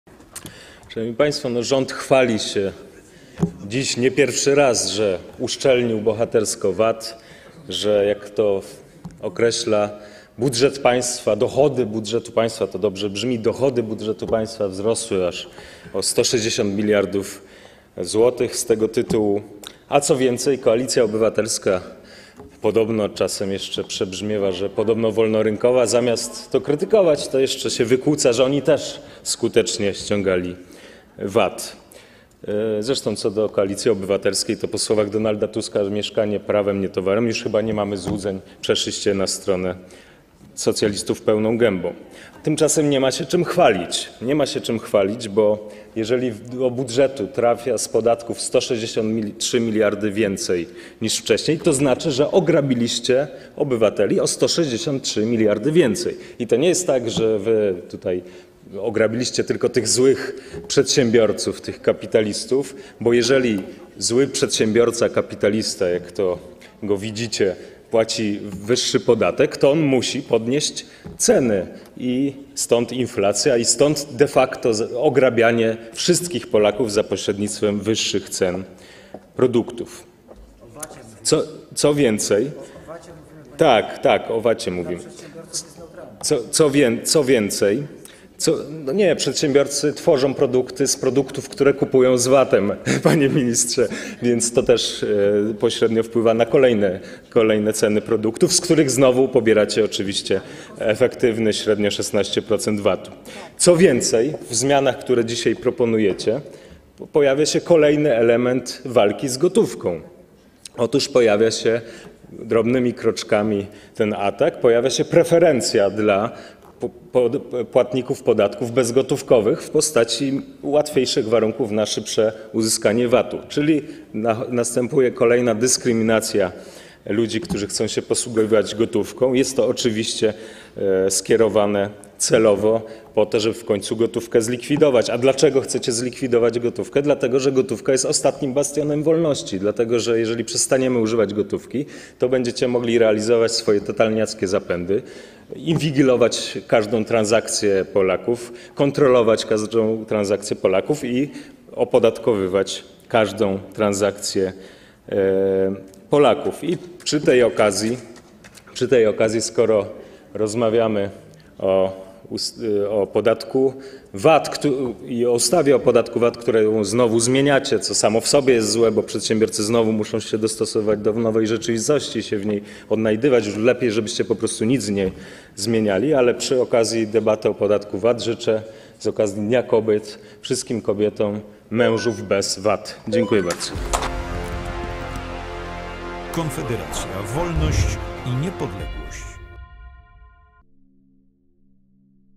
Konrad Berkowicz: Gotówka jest ostatnim bastionem wolności!
Dlaczego rząd chce zlikwidować gotówkę? Dlatego, że gotówka jest ostatnim bastionem wolności! Dlatego, że jeżeli przestaniemy używać gotówki, to rządzący będą mogli realizować swoje totalniackie zapędy bez ograniczeń, inwigilować każdą transakcję Polaków.